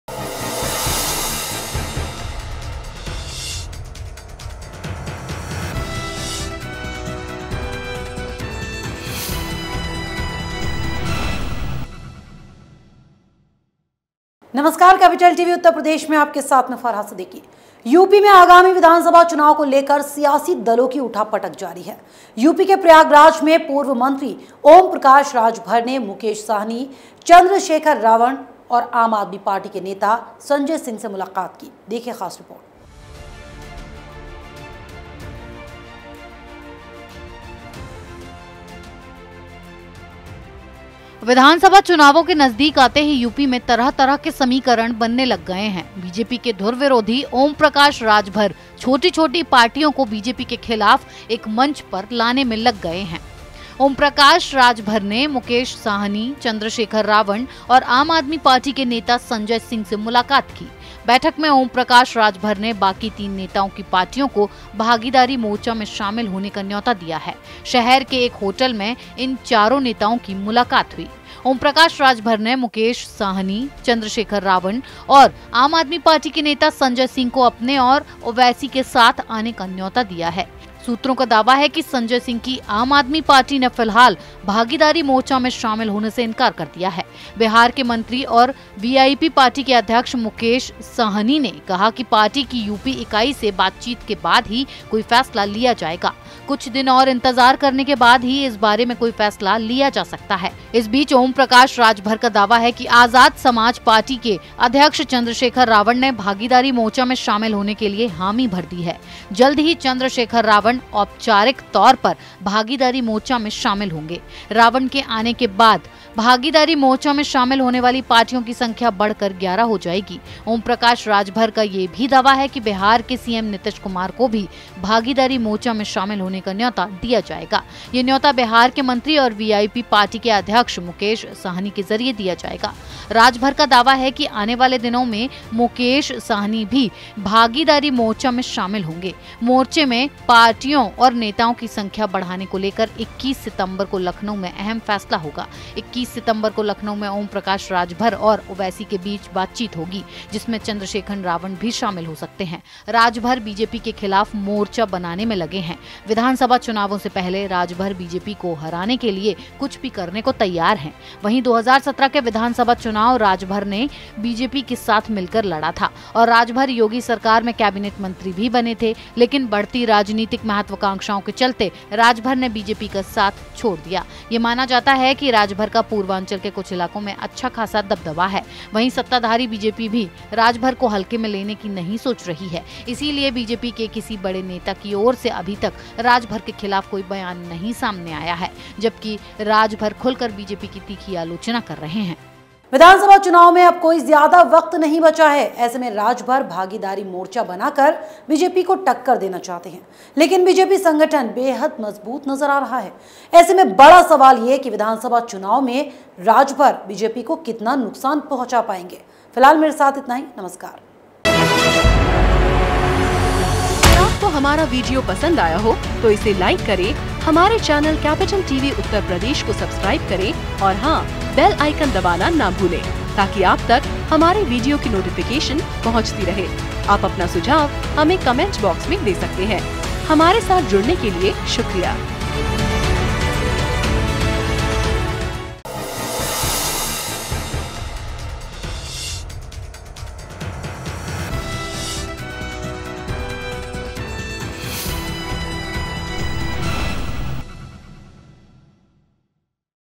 Om Prakash Rajbhar Met These Leaders Including Sanjay Singh | Capital TV Uttar Pradesh
In Uttar Pradesh, the uproar of political parties is going on regarding the upcoming assembly elections. In Prayagraj, UP, former minister Omprakash Rajbhar has met Mukesh Sahni, Chandrashekhar Ravan and Aam Aadmi Party leader Sanjay Singh today. In the meeting, Omprakash Rajbhar has invited the parties of the remaining three leaders to join the partnership front. These four leaders have met in a hotel in the city.